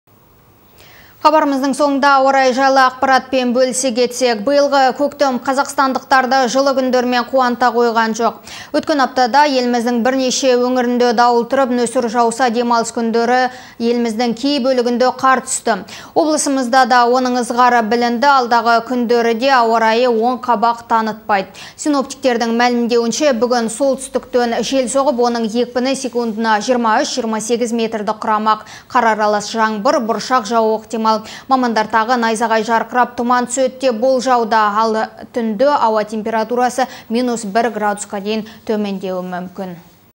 Ауа райы болжамы
Хабарымыздың соңында ауа райы жайлы ақпаратпен бөлісе кетсек. Биылғы көктем қазақтандықтарды жылы күндерімен қуанта қойған жоқ. Өткен жұмада еліміздің бірнеше өңірінде дауыл тұрып, нөсер жауса, демалыс күндері Қазақтанның кей бөлігінде қар түсті